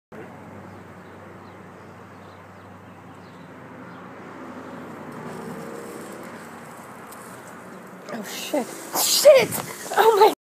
Practice surprise
Almost got hit with the sprinklers.